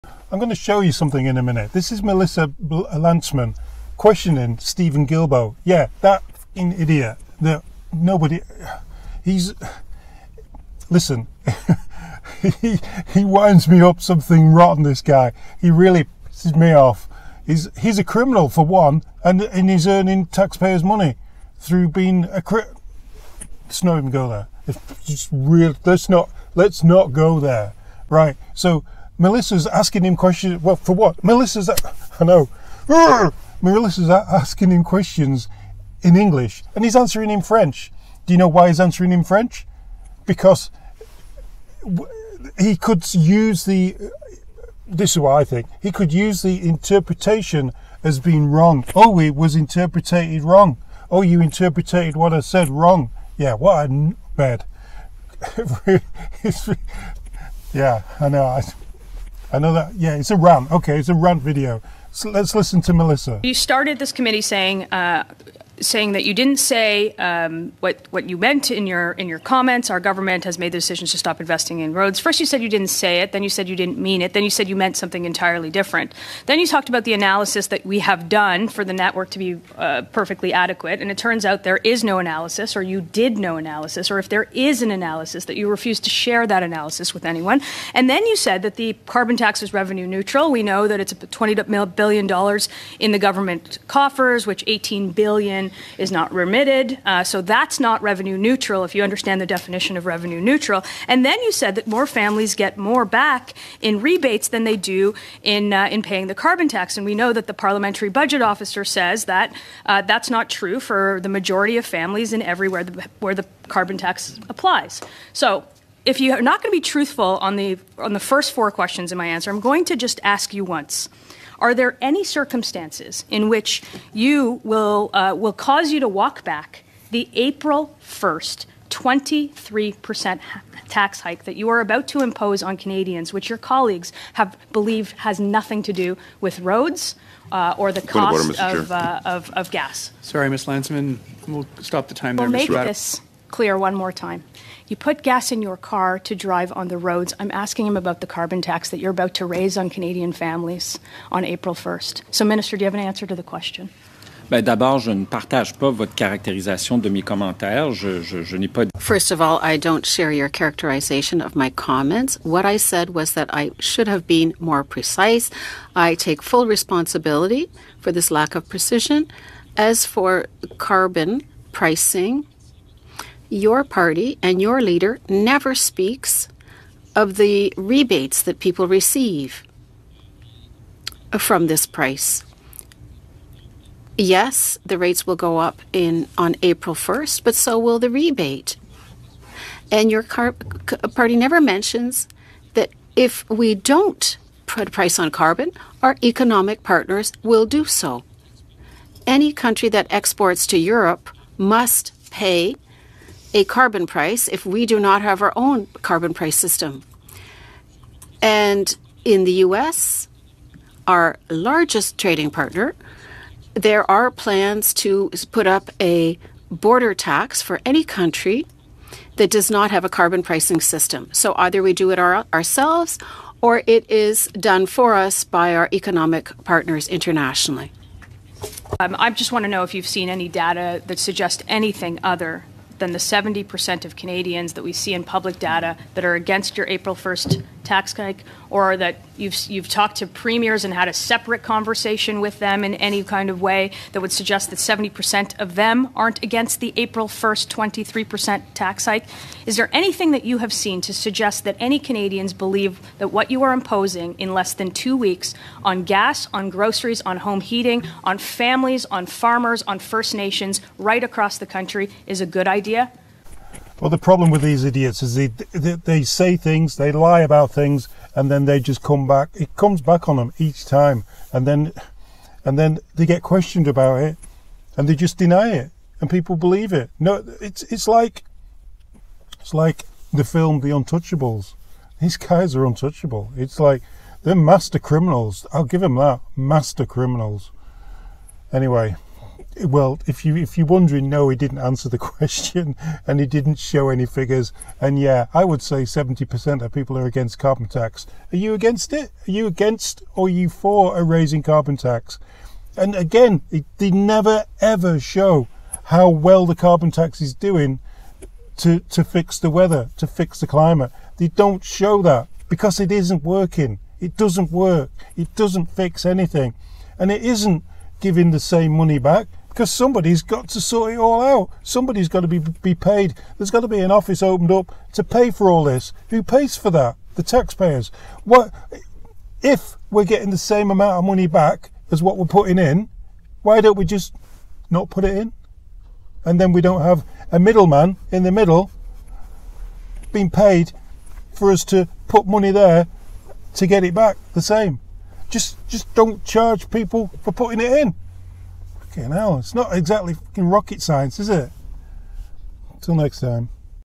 Melissa Lantsman Humiliates Steven Guilbeault Carbon Tax “but” No More Roads
In the inquest into the fact that Steven Guilbeault said he government was not going to invest in any more road infrastructure, Melissa Lantsman humiliates him and he can’t even speak English and speaks French back to her, just so that people can use that as an excuse in the future for his childish answers